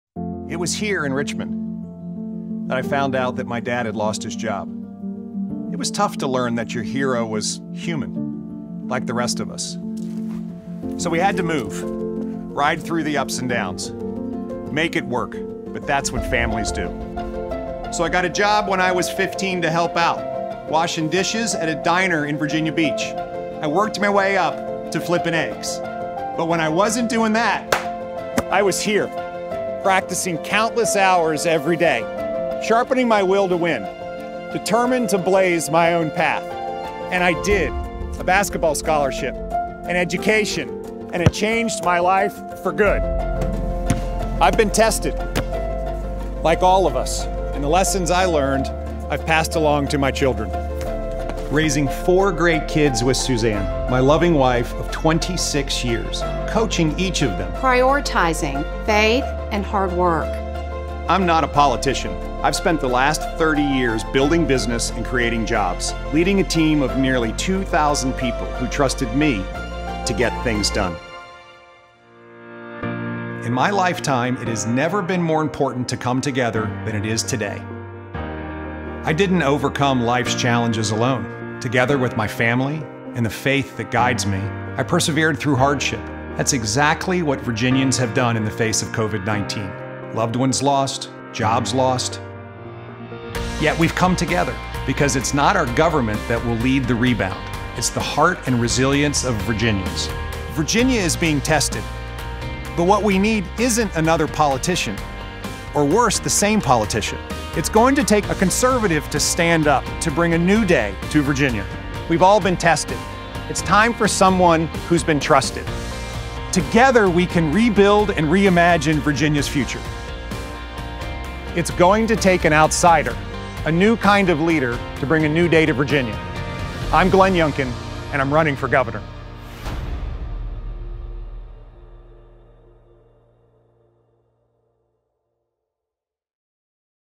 Glenn Youngkin: Tested. Trusted. Together.
“I’m running for governor because I believe we must rebuild and reimagine Virginia’s future and make it the best place in America to live, work, and raise a family. As a homegrown Virginian, I can no longer sit on the sidelines and watch career politicians and insiders in Richmond turn our Commonwealth into California or New York – a place where the cost of living for families is too high and the opportunities for all Virginians to get ahead are too few.

"I’m not a politician, and I certainly don’t have the 120 years of combined political baggage that my opponents have. They talk a lot about solving problems, but I’ve actually done it. I spent the last 30 years building business, creating jobs, and bringing people together to succeed. Guided by my faith, I will bring that same dedication to serving Virginians.

"In the days ahead I look forward to meeting and talking with folks in every corner of the Commonwealth. I’ll be listening to their concerns and sharing my vision for a new day in Virginia.”

Follow Glenn's other social media accounts for the latest updates on his campaign.